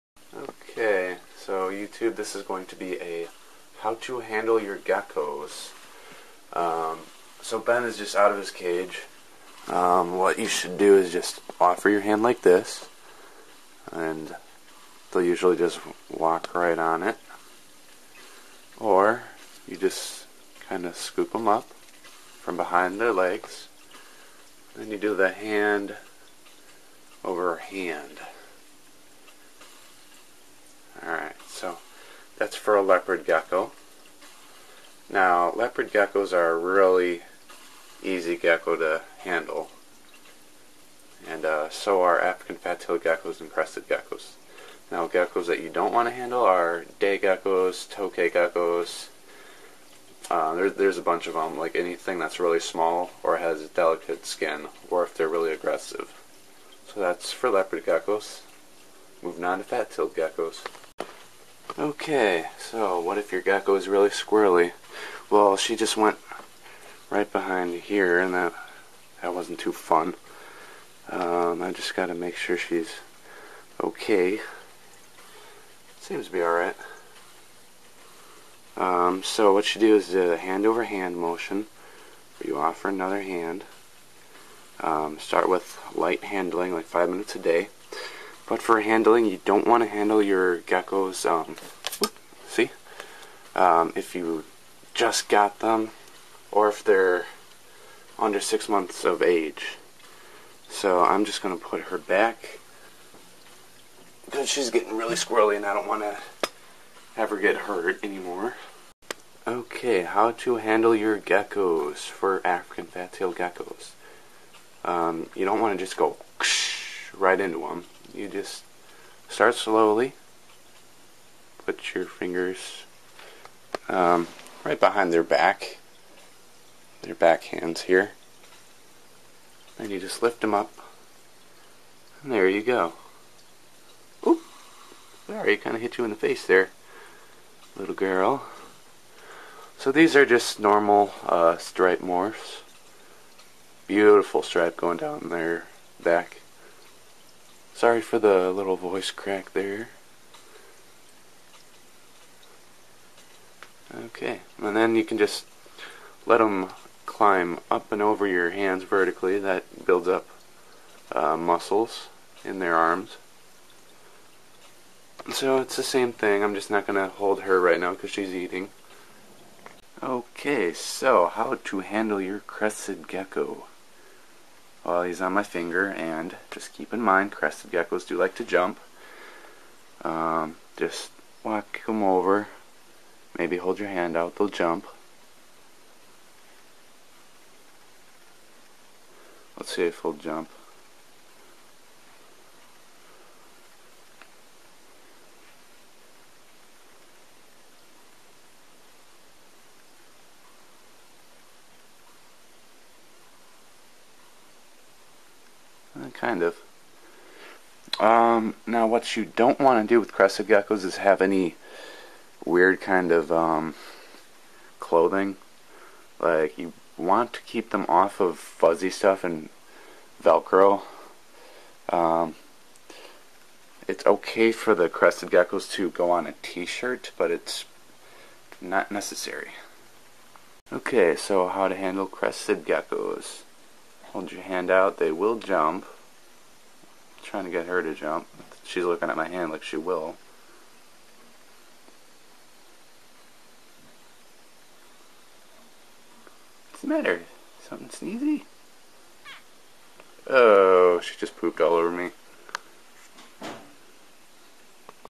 the best starter geckos to keep
here are some of the best started common geckos to get introduced to the hobby

For all cartoon videos i give full credit to the original content creators.